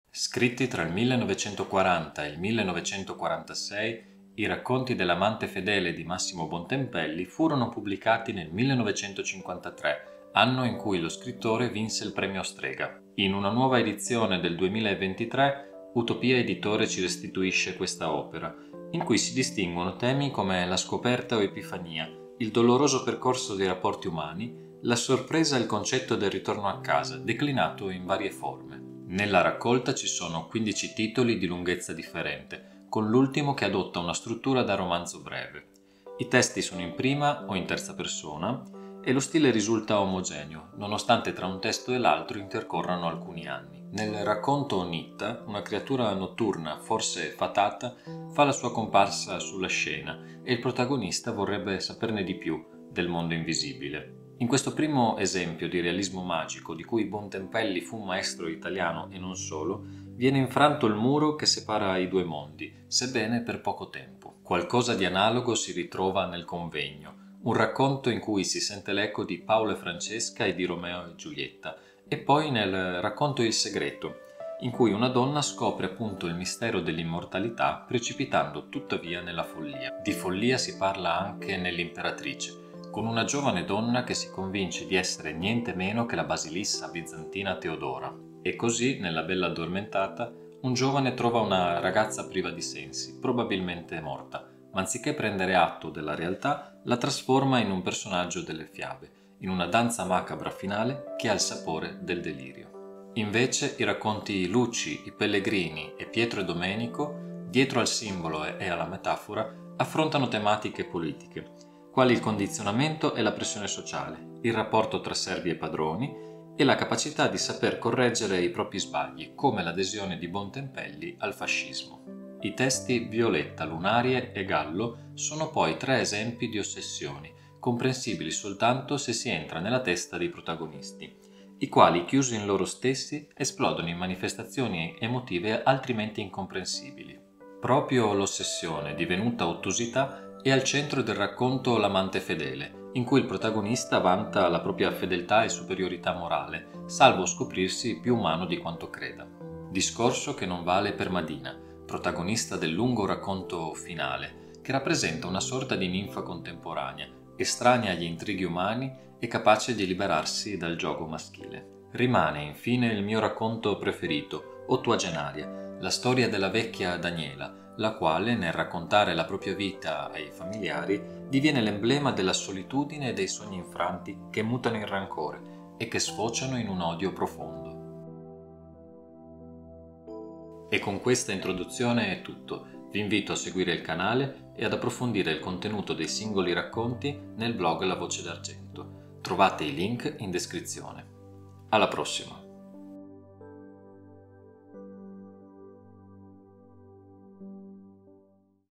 L'amante fedele di Massimo Bontempelli
Scritti tra il 1940 e il 1946, i racconti de "L’amante fedele" di Massimo Bontempelli vennero pubblicati nel 1953, anno in cui lo scrittore vinse il Premio Strega.

Quindici titoli, di lunghezza differente, con l’ultimo racconto che adotta una struttura da romanzo breve e una suddivisione in capitoletti. Un’altra distinzione riguarda il passaggio dalla prima alla terza persona, tuttavia, pur intuendosi come i testi siano stati scritti in diverse fasi, lo stile risulta omogeneo. Un tratto che mi ha colpito, e che mi ha riportato alla mente gli scritti italiani dell’epoca, è l’alternanza dei tempi verbali passati con quelli presenti, anche all’interno dello stesso periodo, e l’utilizzo del passato remoto laddove oggi si preferisce il passato prossimo.

Bontempelli è poi il maestro del realismo magico all’italiana e la maggior parte dei racconti de "L’amante fedele" lo dimostra. In una nuova edizione del 2023, Utopia Editore ci restituisce questa opera in cui si esaltano temi come la scoperta, o epifania, il doloroso percorso dei rapporti umani, la sorpresa e il concetto del ritorno a casa, declinato in varie forme. Di seguito, riporto alcune considerazioni sui racconti e, trattandosi ormai di un classico, ne scriverò non senza alcuni spoiler.

"Nitta"

Nitta è una creatura notturna, non sembra fatata, ma più un’anima in pena che non può fuggire la notte. Il racconto è narrato in prima persona da un uomo alla guida di un’automobile. Egli sente rumori e sospiri nella vettura e vi scopre una ragazza addormentata. I due vagano per le strade, l’uomo è sempre più incuriosito da lei: «“Le “presenze invisibili” farneticavano accelerando: ricordo che da ragazzo per un certo tempo questa frase mi piacque molto. È un peccato che al ginnasio non mettano in programma un poco di demonologia, avrei imparato qualche esorcisma. Ma chi può sapere se questa presenza sia maligna o benigna?»

Bontempelli sarebbe forse stato contento di scoprire che oggi, nel Regno Unito, esiste un master in scienze magiche e occulte. Il mistero intorno a Nitta rimane comunque tale. La creatura svia le domande e alla prima occasione scompare. Prende avvio il gioco delle prospettive, o dei punti di vista, che segna molti racconti della raccolta. Infatti, ciò che per il narratore è un’incredibile epifania, potenziale fonte di significato esistenziale, per Nitta è una notte qualsiasi, forse persino più inutile del solito.

"Il ladro Luca"

Qui siamo dalle parti dei dilemmi morali. Un ladro è in fuga su un tetto, un poliziotto lo raggiunge, ma scivola e rischia di precipitare: il ladro Luca decide di salvarlo e se ne va con la refurtiva.

La domanda è: il poliziotto viene salvato perché Luca teme di essere accusato di omicidio? Oppure è un autentico gesto d’umanità? Non basta questo passaggio: «D’improvviso qualche cosa d’ignoto brillò nell’animo del ladro Luca, ed era assai diverso dal delirio di quella prima felicità.»

Sì, perché sulle prime, vedendolo scivolare, l’uomo aveva riso della sua sorte. L’istinto si manifesta come primo motore dell’azione, ma – a pensarci bene – il senso di umanità prevale. A volte, il caso stimola le persone a una buona azione prima che sopraggiunga il rimorso dell’irrevocabile. Non sempre, però, abbiamo questa fortuna.